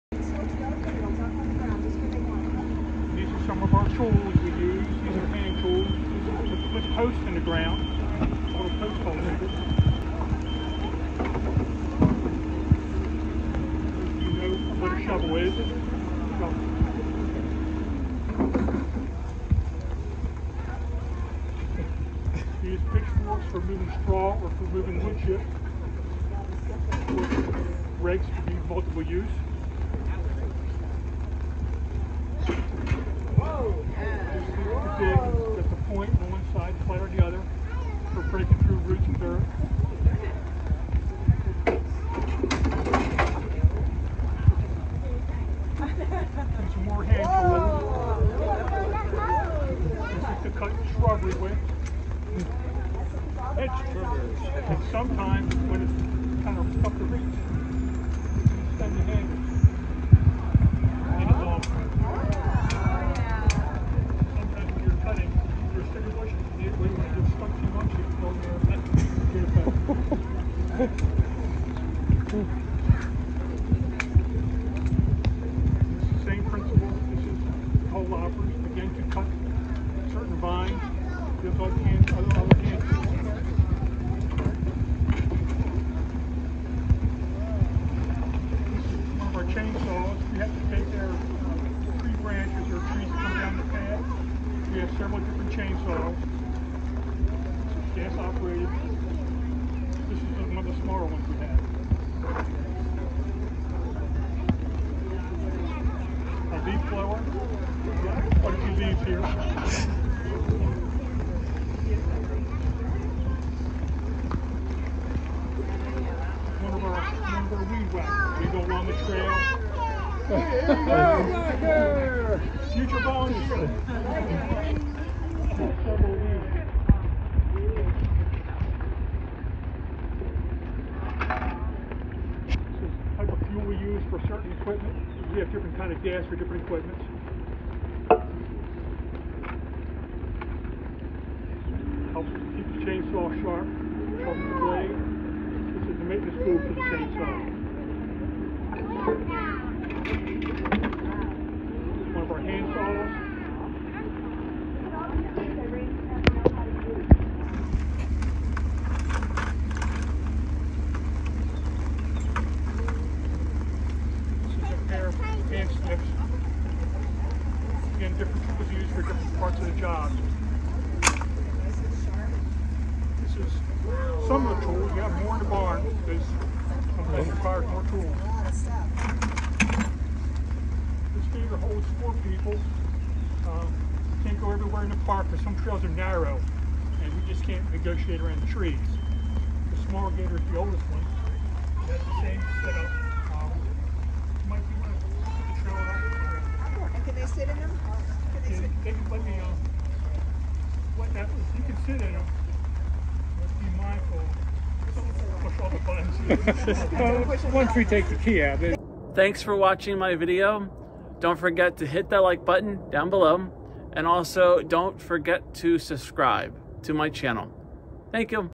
Tots's program Day 1 at Cromwell Valley Park
Today we let the kids check out the tools we use at the park. I think it's called the Tots's program. It was a two day program with different kids.